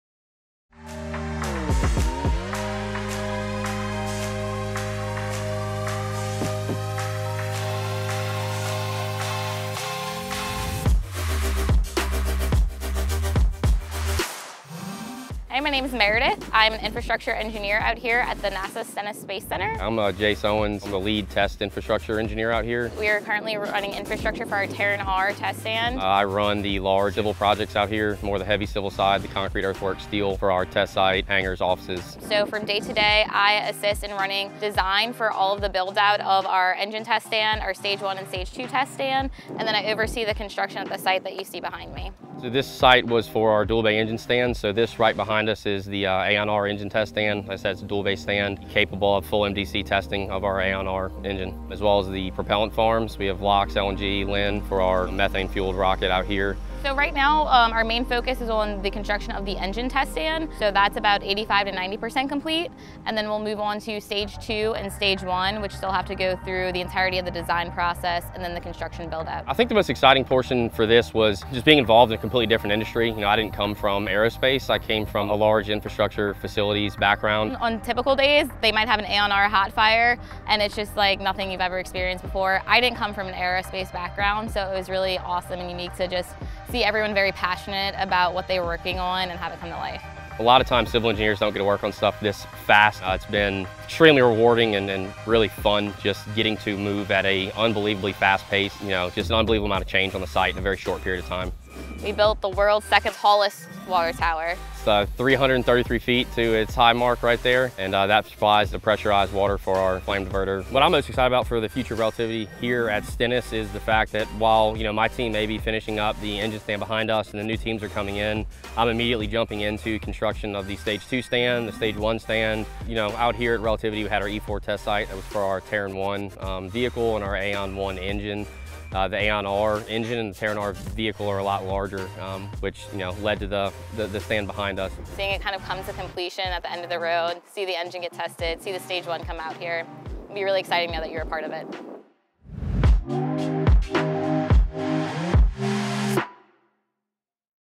📍R Complex Build Update, Stennis, MS
Our Senior Manager, of Infrastructure and Infrastructure Engineer talk R Complex buildout, day-to-day work and what’s on the horizon. 🚀

✔️ Dual Bay engine test stand for Aeon R capable of full MDC testing
✔️ Propellant farms & one of the world’s tallest water towers at 333ft.
✔️ Stage 2 and stage 1 test stands